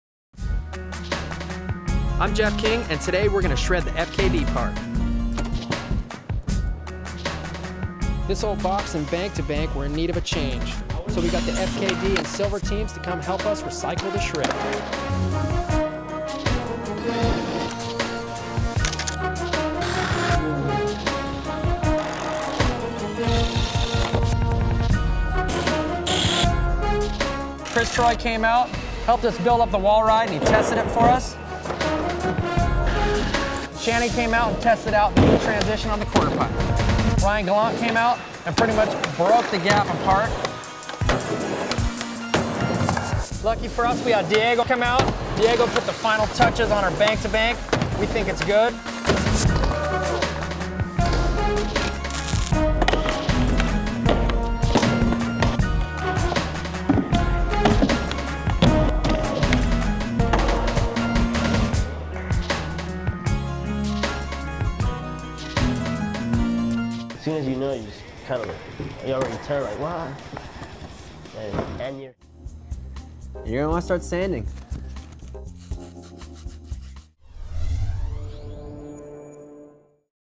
Jeff King Rebuilds the Syndrome Park - FKD Recon Ep. 1
FKD bearings and Silver Trucks invite Jeff King to come and re build their park. Jeff does a quick make over with some help from Josh Kalis before the team rips the park.